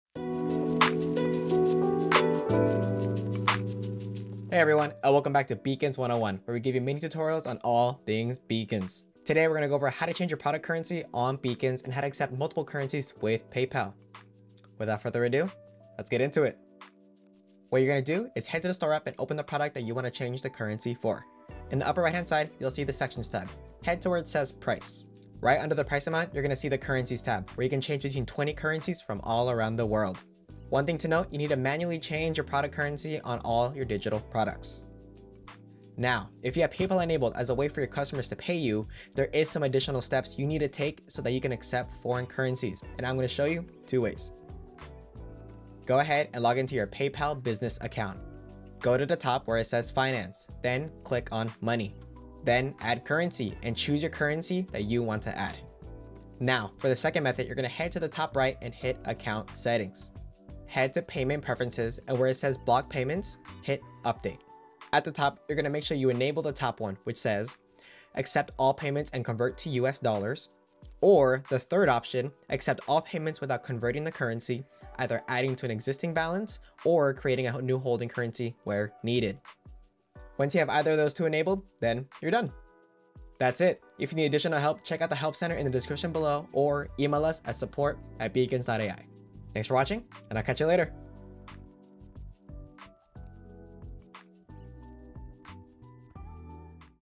Setting Up Multi Currency with Beacons and Paypal (Tutorial)
Welcome back to Beacons 101 where we give you quick tutorials with Tips & Tricks on all things Beacons!

Todays tutorial is how to change your currency on Beacons as well as how to enable multi-currency payments with PayPal so that all your fans from around the world can buy your products!

🕐  Timestamps  🕐

0:00 - Introduction 🎬
0:17 - Changing Your Product Currency on Beacons 💰
0:36 - Enabling Multiple Currencies on PayPal 💴
0:45 - Method 1️⃣
0:56 - Method 2️⃣
1:24 - Ending 🏁